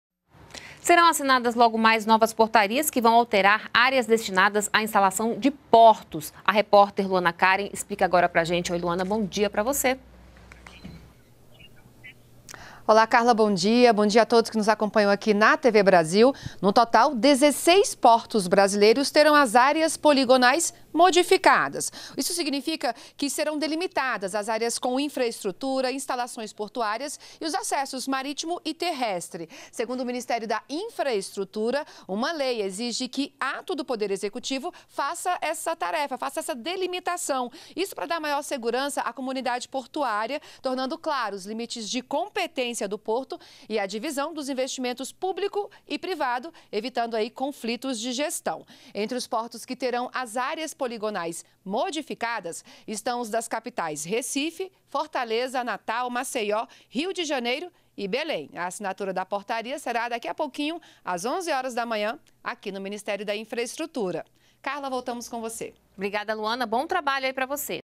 Portarias que alteram áreas destinadas a instalação de portos serão assinadas hoje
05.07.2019 - Portarias que alteram áreas destinadas a instalação de portos serão assinadas hoje. No total, 16 portos brasileiros terão as áreas poligonais modificadas. Serão delimitadas as áreas com infraestrutura, instalações portuárias e acessos marítimos e terrestres.